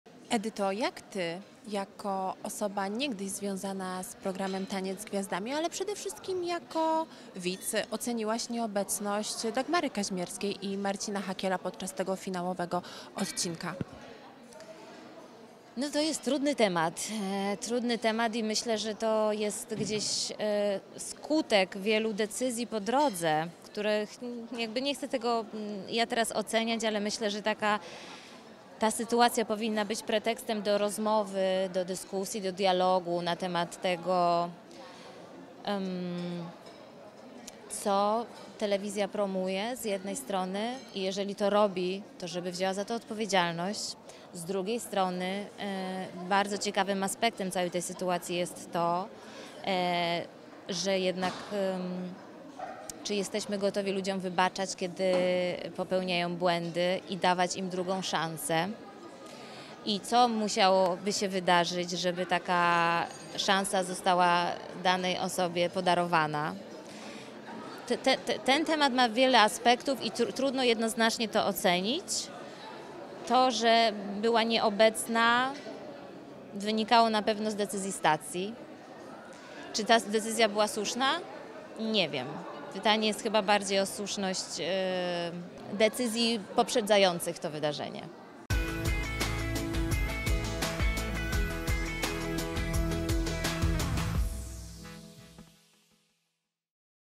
Edyta Herbuś o nieobecności Kaźmierskiej i Hakiela podczas finału "TzG"
Edyta Herbuś o nieobecności Kaźmierskiej i Hakiela podczas finału "TzG". Więcej w naszym materiale wideo.